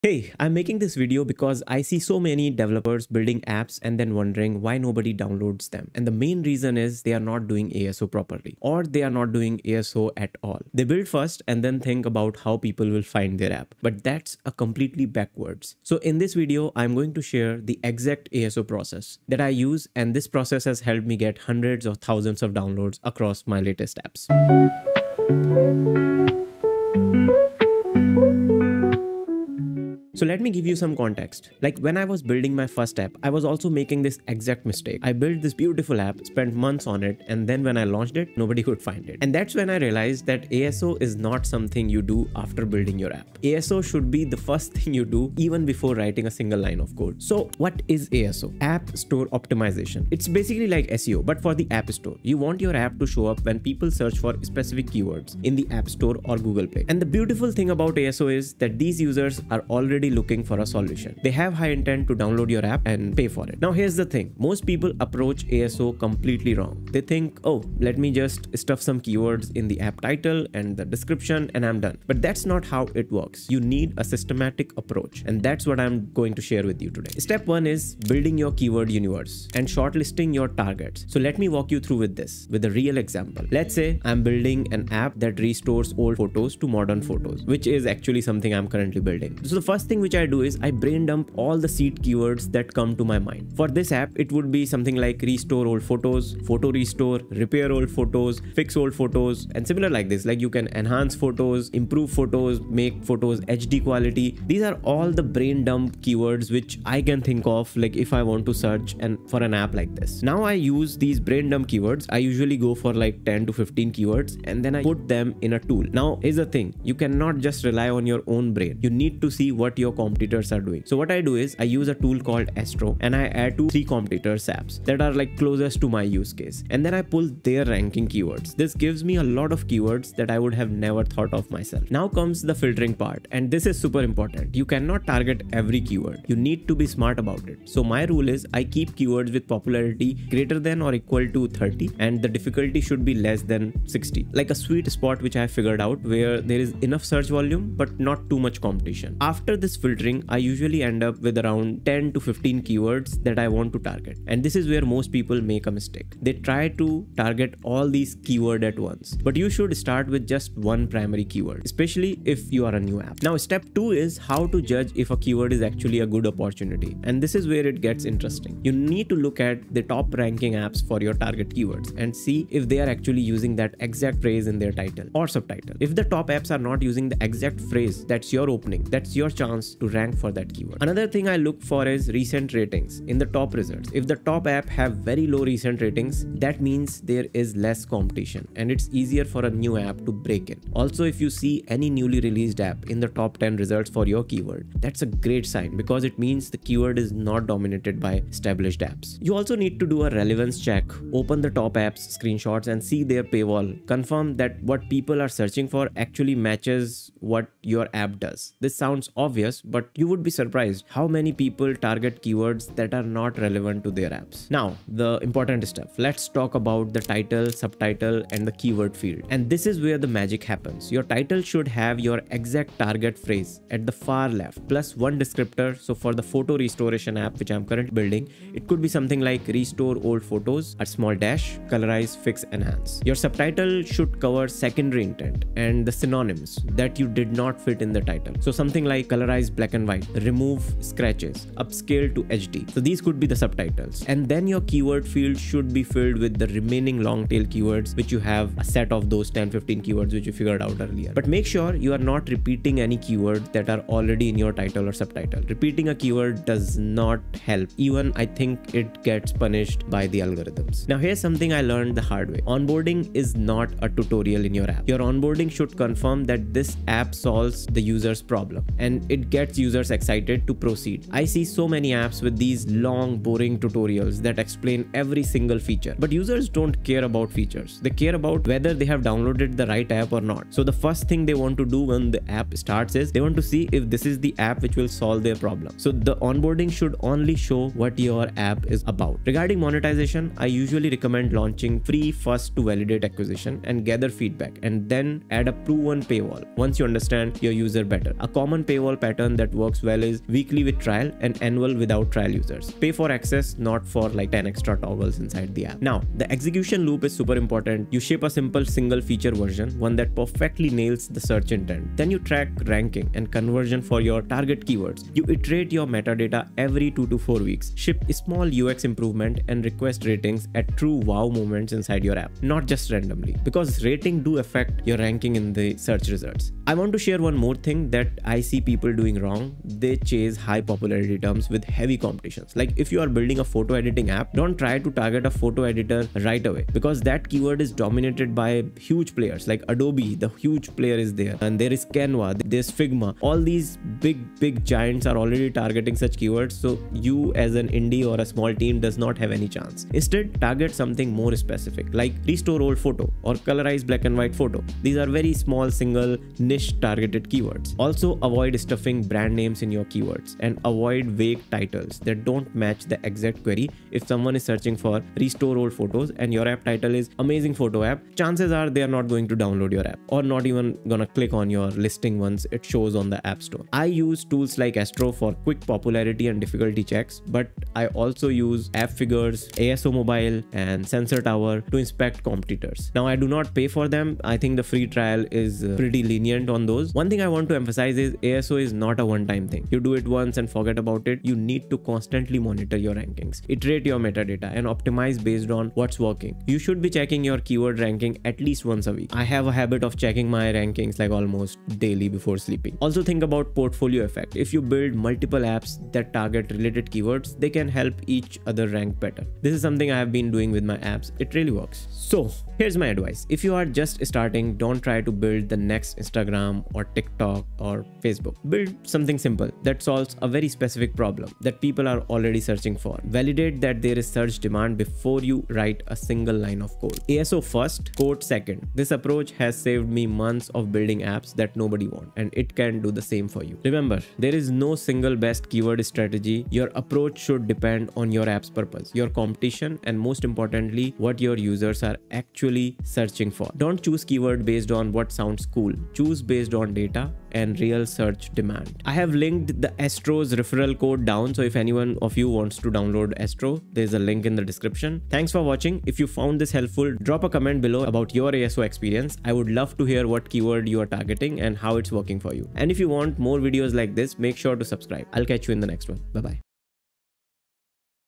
My Simplest ASO Strategy to get to $10k MRR
I see so many developers ship great apps and then wonder why nobody downloads them. I did the same thing with my first app. I built first and thought about ASO later. Big mistake. In this video I share the exact ASO (App Store Optimization) process I use today to rank on the iOS App Store and Google Play and bring in hundreds or even thousands of organic downloads.